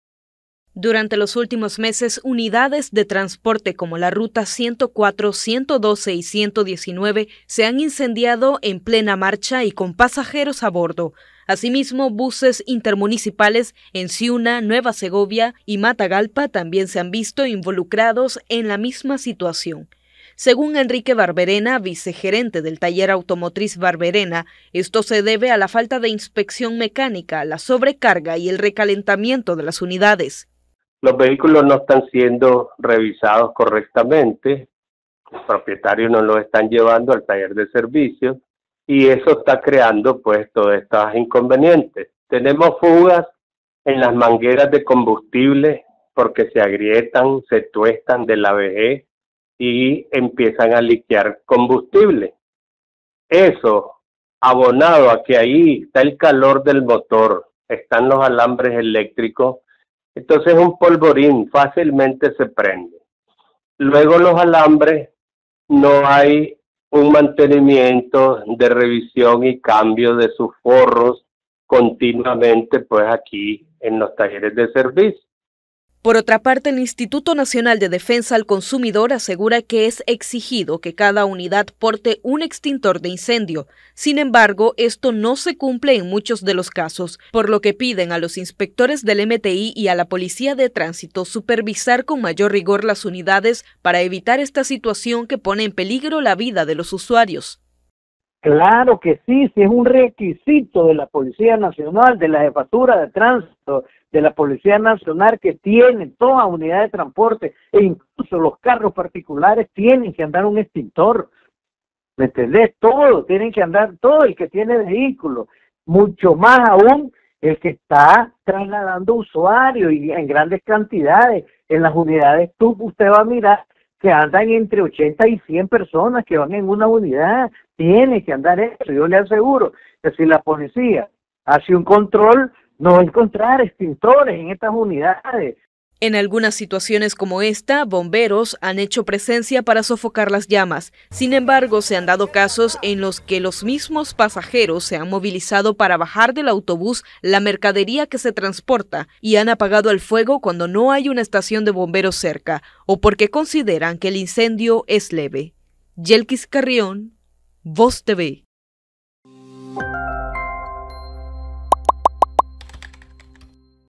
Incendios en rutas y buses intermunicipales son cada vez más comunes
Durante los últimos meses, unidades de transporte como la ruta 104, 112 y 119 se han incendiado en plena marcha y con pasajeros a bordo, asimismo, buses intermunicipales en Siuna, Nueva Segovia y Matagalpa, se han visto involucrados en la misma situación, según Enrique Barberena, vicegerente del taller automotriz Barberena, esto se debe a la falta de inspección mecánica, la sobrecarga y el recalentamiento de las unidades.